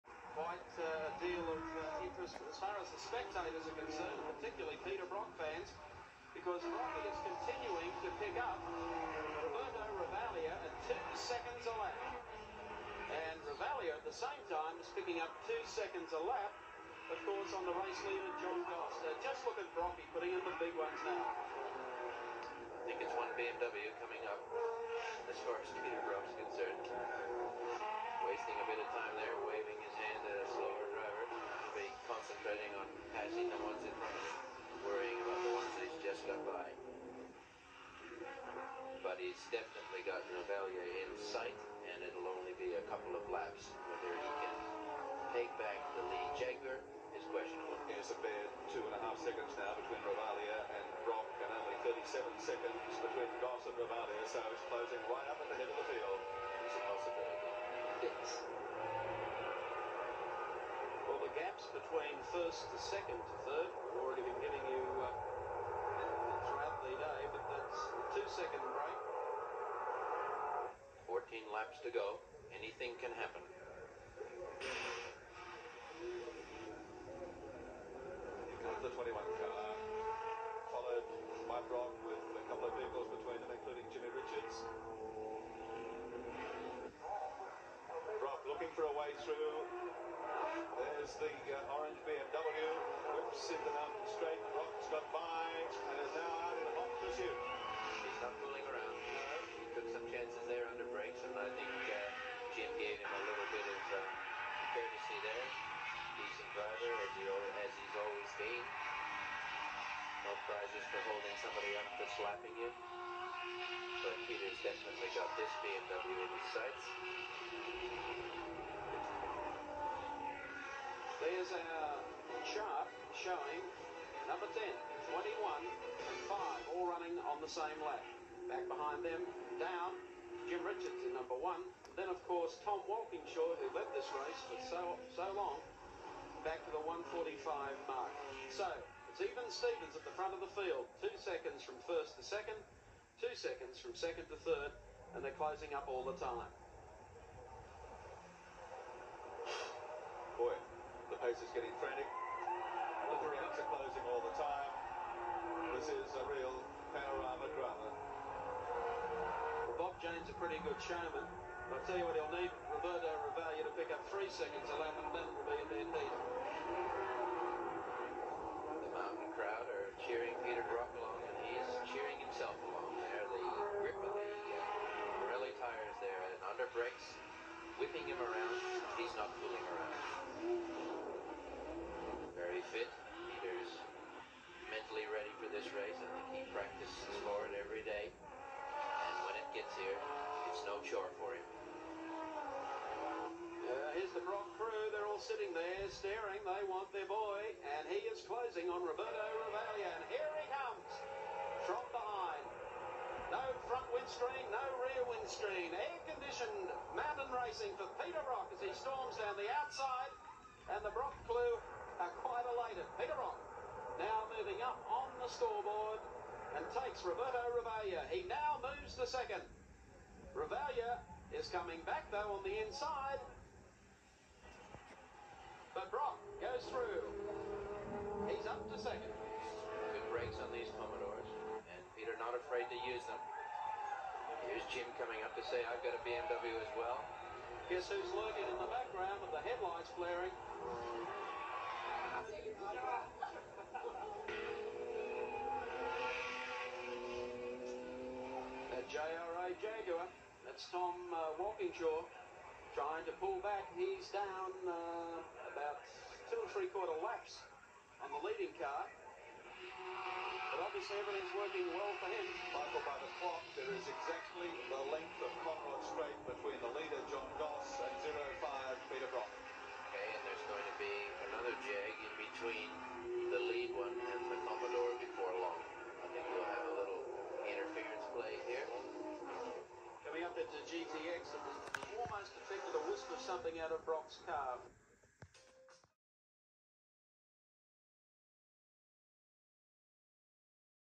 Peter Brock’s Bathurst 1985 Skills Were Beyond Genius
Getting content off my old VHS collection of the legend Peter Brock